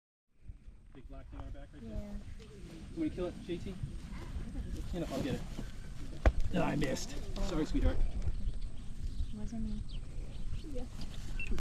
pesky bug biting a llama
Clover Brooke Farm in Hyde Park, New York.